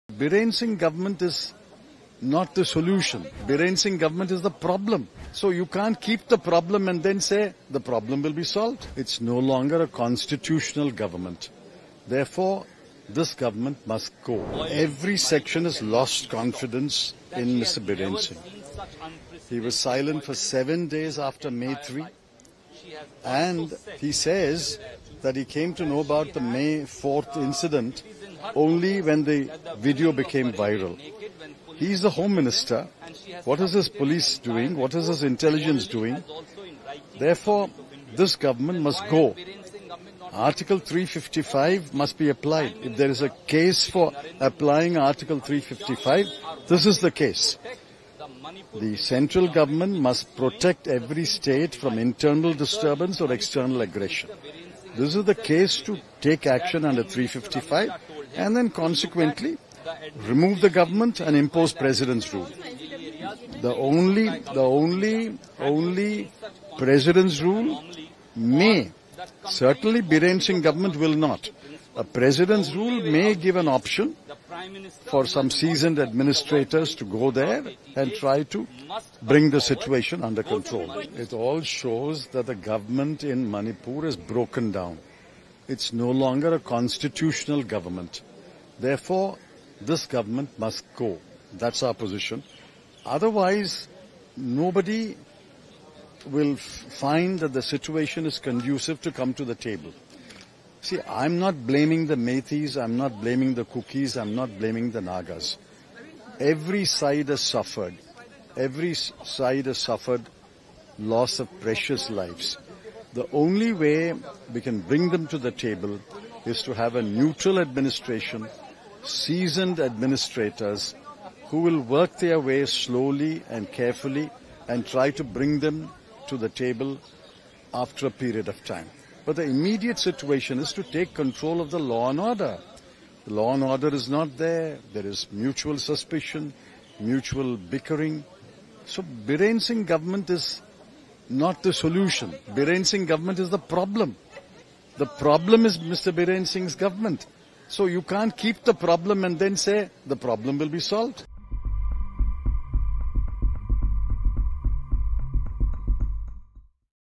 P.Chidambram, congress leader demand CM Manipur to resignation immediately.
Video from Dosh Dasi Tamin